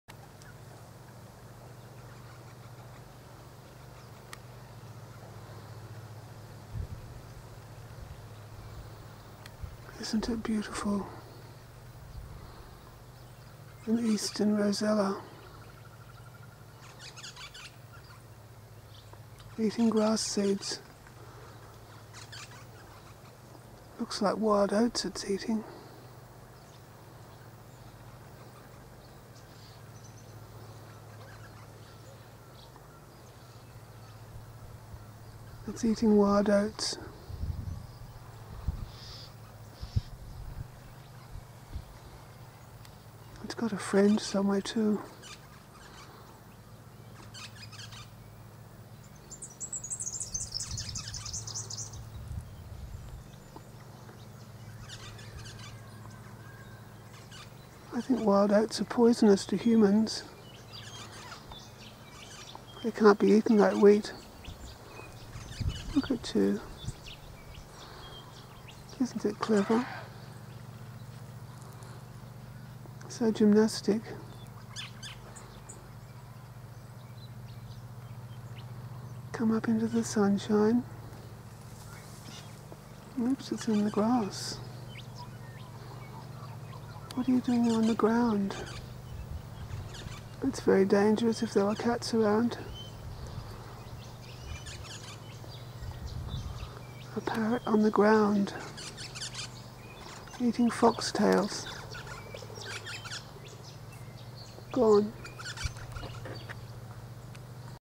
Rosella Eating Wild Oats (•▽•)
Here is a beautiful Eastern Rosella (Platycercus eximius) eating wild oats and foxtail seeds. In Australia we always have pretty parrot visitors to brighten up the place with their comic-book-cover colourfulness.

(•▽•)ლ༼( ͡°╭͜ʖ╮͡ ͡°)༽